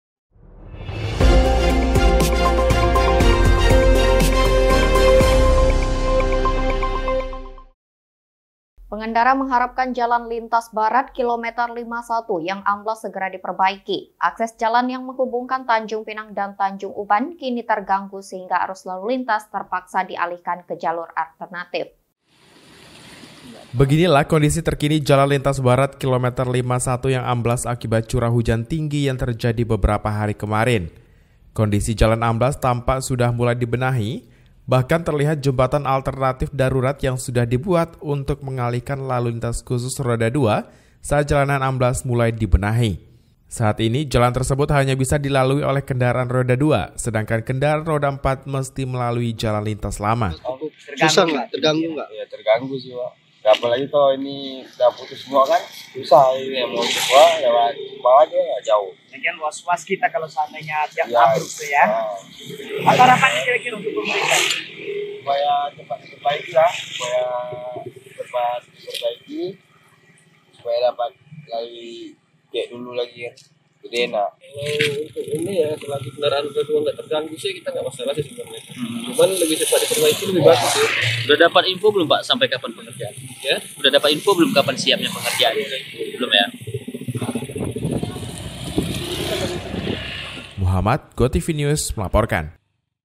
Pengendara Harap Jalan Amblas di Lintas Barat KM 51 Segera Diperbaiki
GOTVNEWS, Bintan - Pengendara mengharapkan Jalan Lintas Barat KM 51 yang amblas segera diperbaiki. Akses jalan yang menghubungkan Tanjungpinang dan Tanjung Uban kini terganggu sehingga arus lalu lintas terpaksa dialihkan ke jalur alternatif.(Mhd)